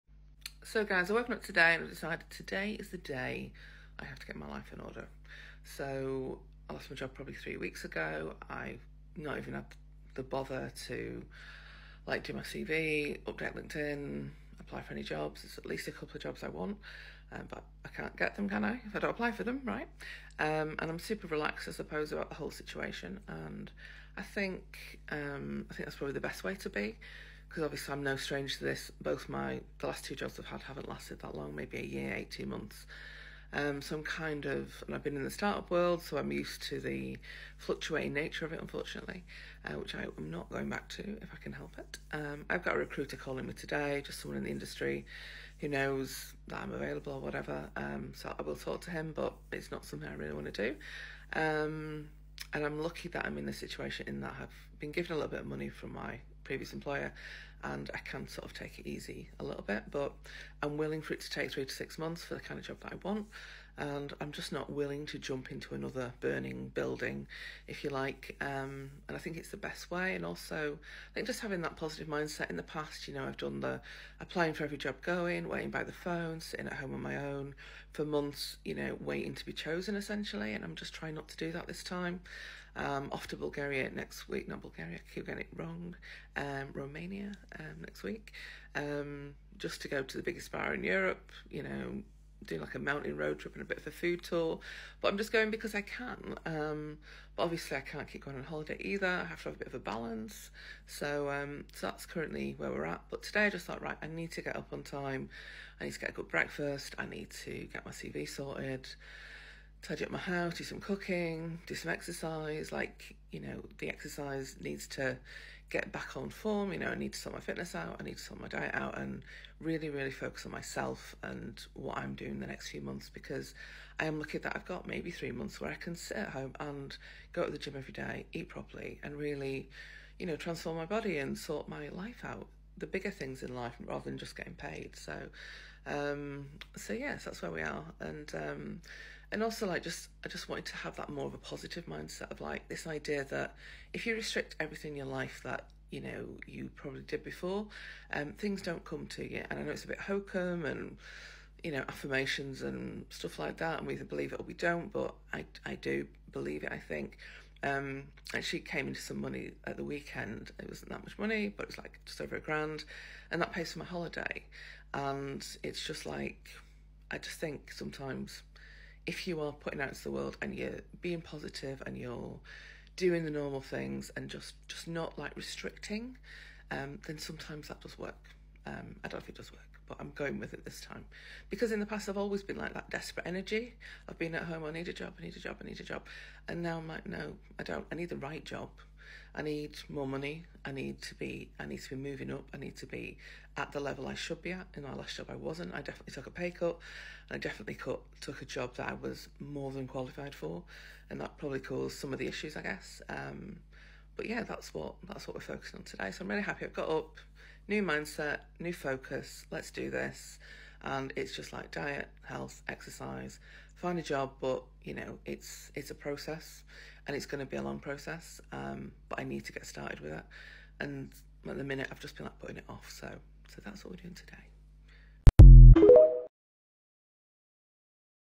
5 January 2024
First day of my new life! Trying to focus and get my life in order! New job needed, need to focus on next steps.